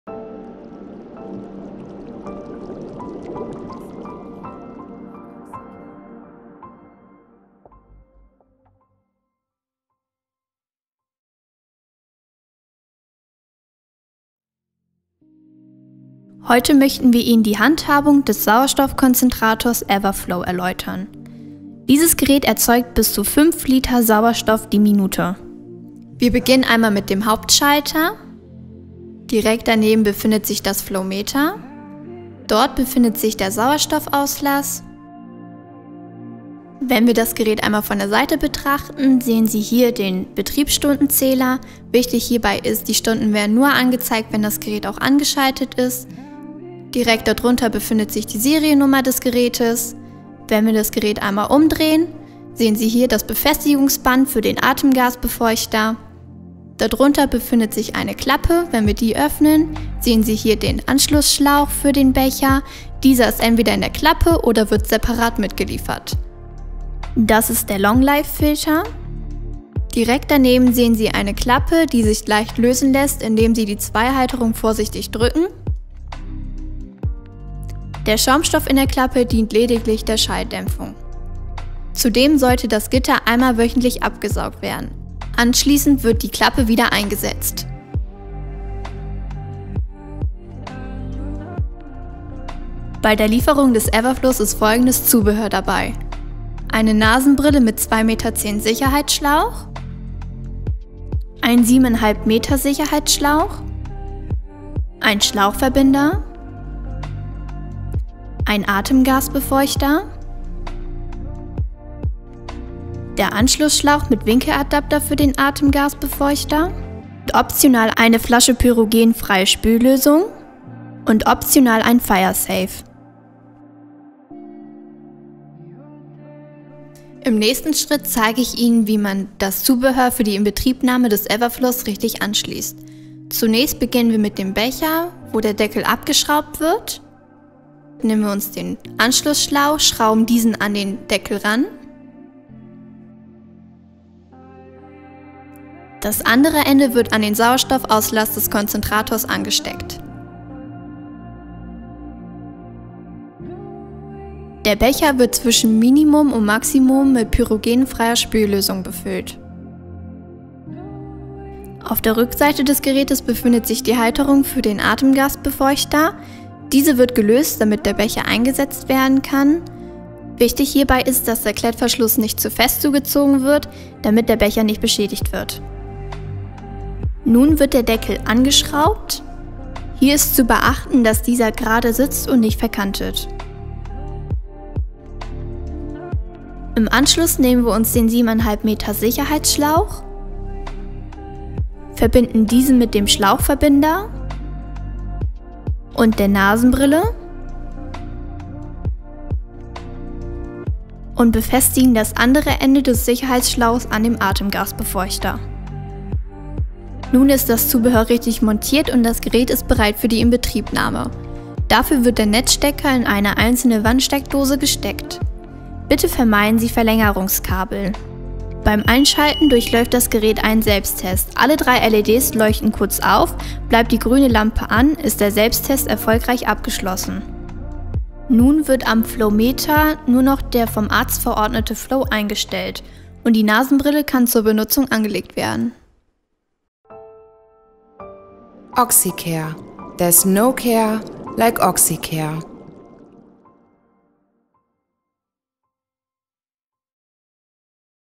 EverFlo Sauerstoffkonzentrator OxyCare Medical Group: Geräteeinweisung
Dieses Video zeigt, wie Sie den stationären Sauerstoffkonzentrator EverFlo bedienen: Vom Anschluss des Zubehörs bis zur Inbetriebnahme.

Kapitel
0:00 Sicherheitshinweise Sauerstoffkonzentrator
0:16 Geräteübersicht (vorne, seitlich + hinten) & Erklärung
1:33 Zubehör (Erstausstattung + optionales Zubehör)
2:07 Anschluss vom Zubehör & Inbetriebnahme Everflo